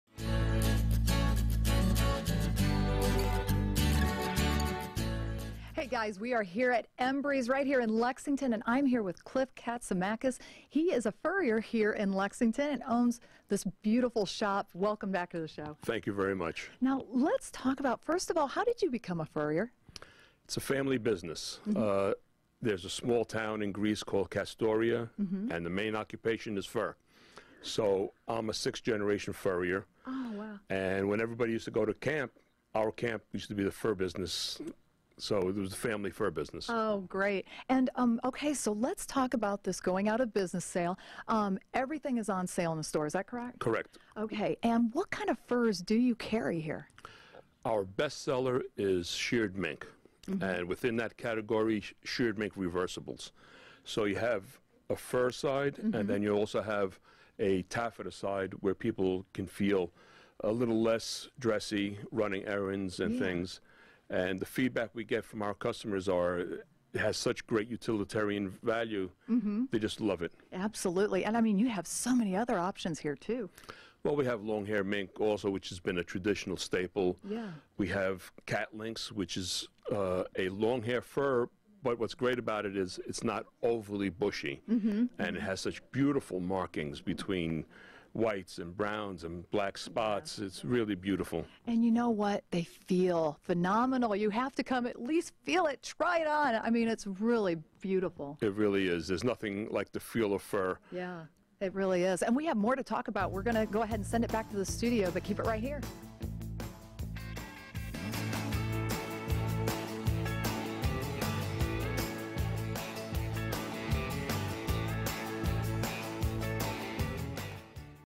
Embry's Fur
Embry's Fur in Lexington is going put of business. Tune in and hear about all they have to offer.